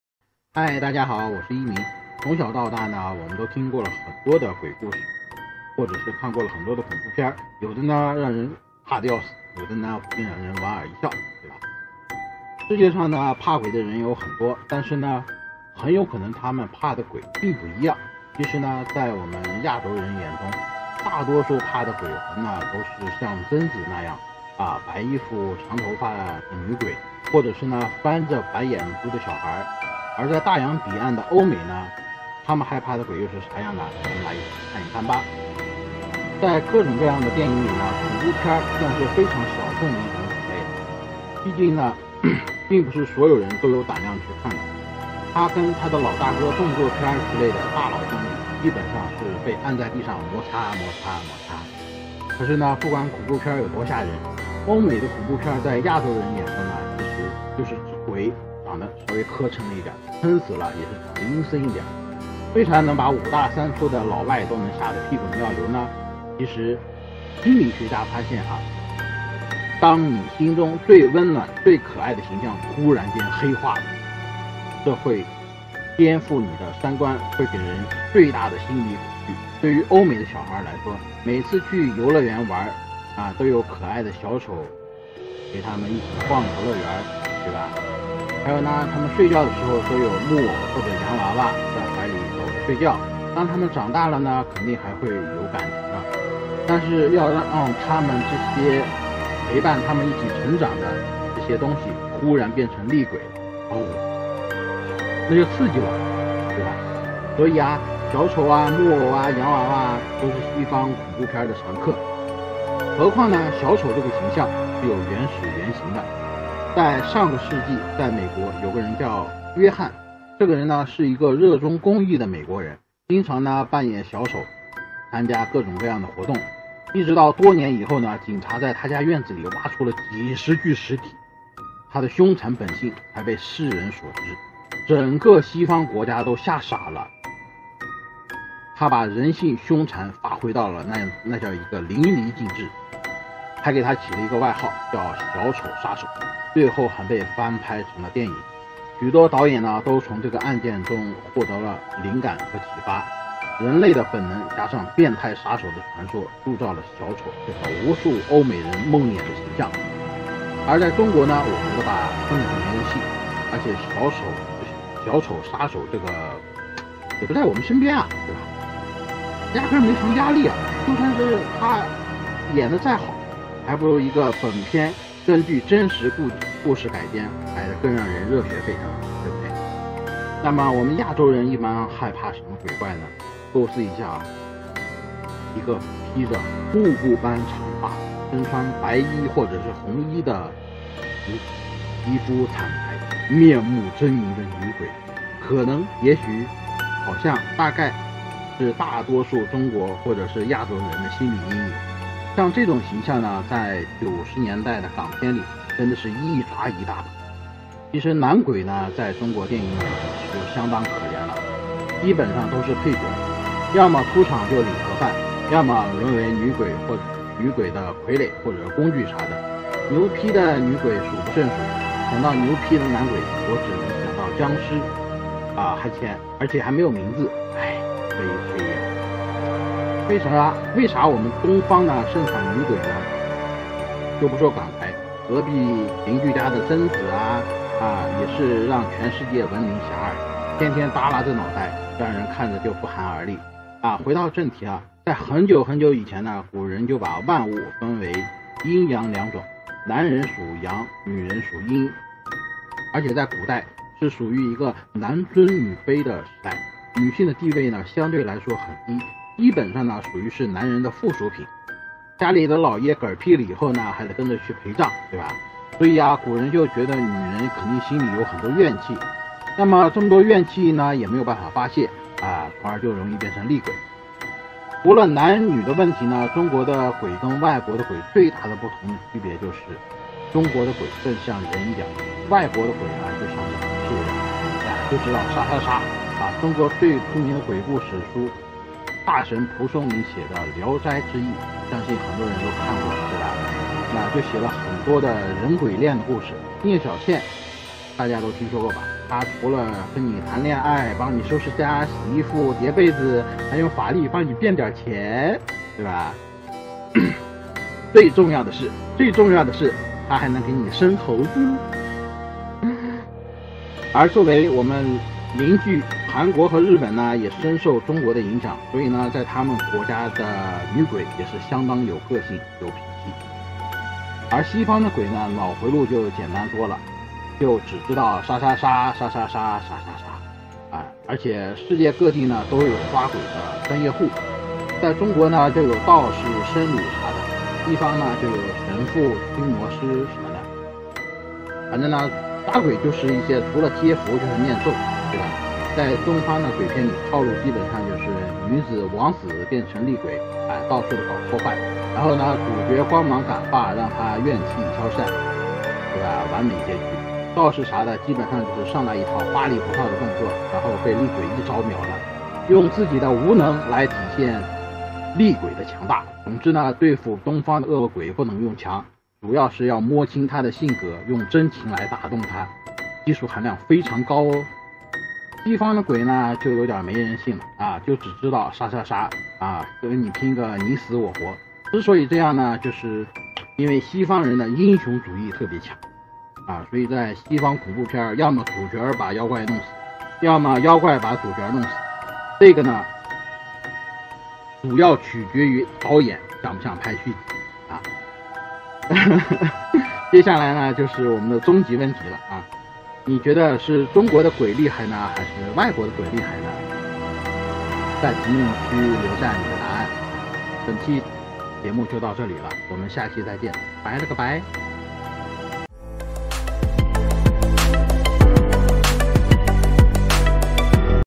【都市传说】到底是中国的鬼厉害呢还是外国的鬼厉害？
从小到大，我们都听过许多的鬼故事，看过很多的恐怖片，有的怕的 要死，有的莞尔一笑，世界上怕鬼的人很多，但是呢，有可能他们怕的鬼可能并不一样，其实，在亚洲人眼中，大多数怕的鬼都像贞子那样的白衣女鬼，或者是翻着白眼珠的恐怖小孩，而在大洋彼岸的欧美眼中，他们害怕的又是啥呢？我们来一起看一看。